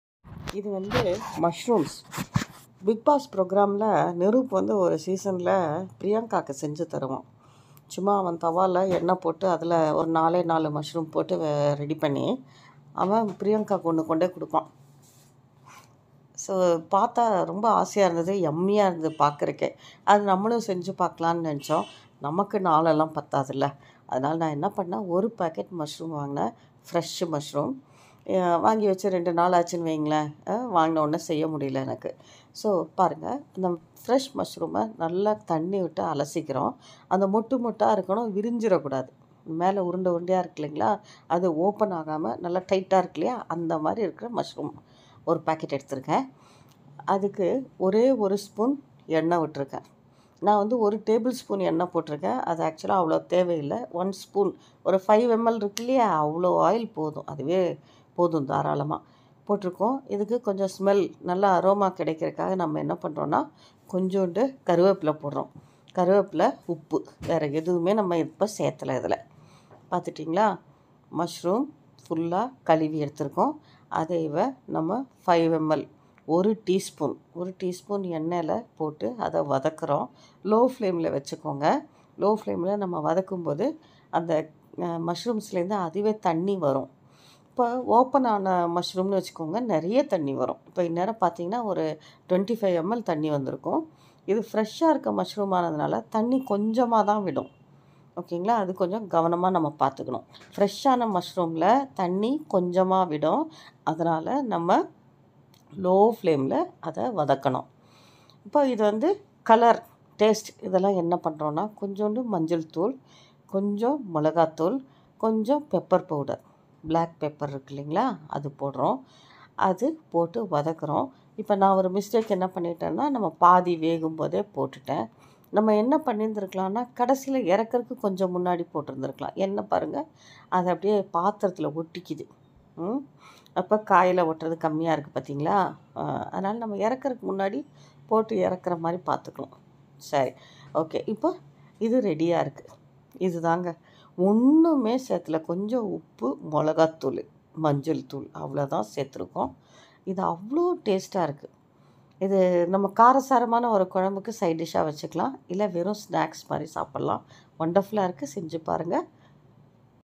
Bigg Boss Niroop's Mushroom Recipe
200gm Mushroom
Saffron, Salt, Chillie powder
A spoon of oil